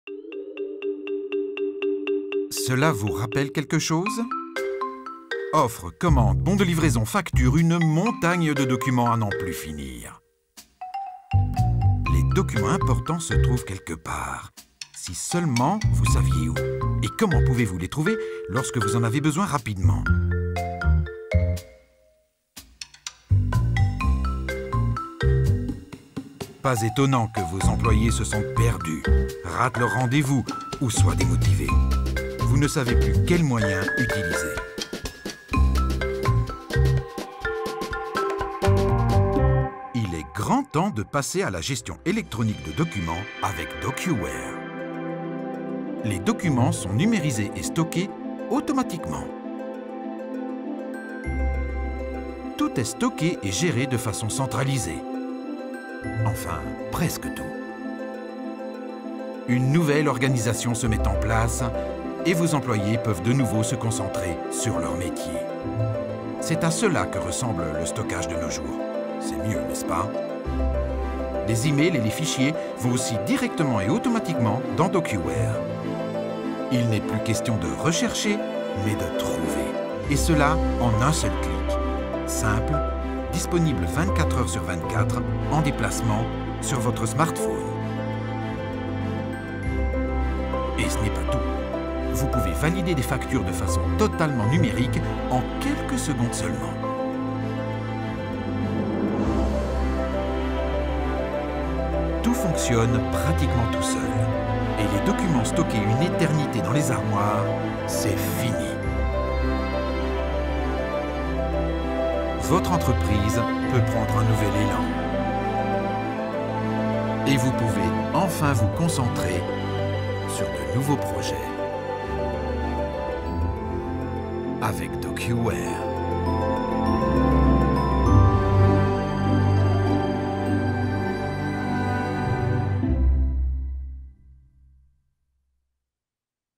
Il est grand temps de passer à la gestion électronique de documents Avec DocuWare !
Cela vous rappelle-t-il quelque chose ? Offres, commandes, bons de livraison, factures. Une montagne de documents à n'en plus finir !

Les documents importants se trouvent quelque part si seulement vous saviez où et comment pouvez-vous les trouver lorsque vous en avez besoin rapidement ?

Pas étonnant que vos employés se sentent perdus ratent leurs rendez-vous ou soient démotivés.

Vous ne savez plus quels moyens utiliser ! Il est grand temps de passer à la gestion électronique de documents Avec DocuWare !